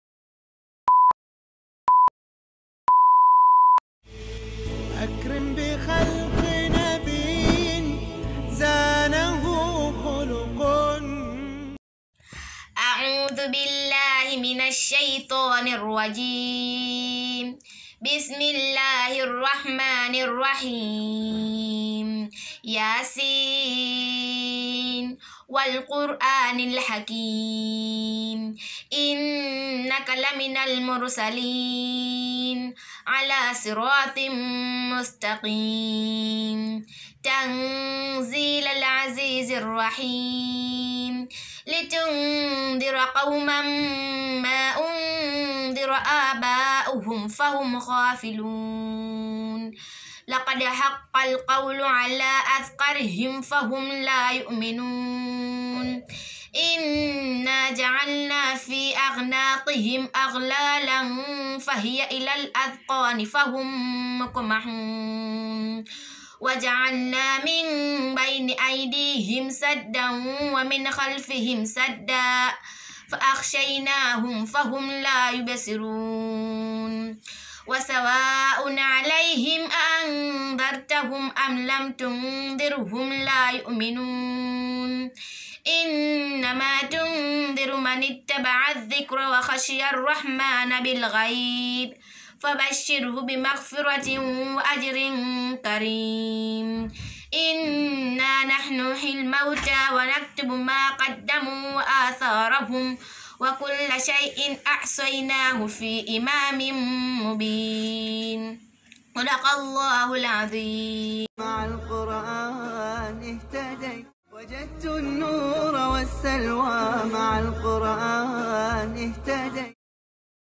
Mshiriki namba 040 SALMA ALI MOHAMED kutoka ZANNZIBAR
Washindi watapatikana kwa njia mbili
1. Majaji watatu watazipitia clip zote kuangalia hukmu za Quran
2. Chaguo la watazamaji  kukoment kwa kuandika jina la mshindi kwa wingi.

Kukoment ingia kwenye Youtube Chanel ya ibunjazarTV andika unayemuhisi wewe kuwa ni mshindi na sisi tutahesabu kura za washindi.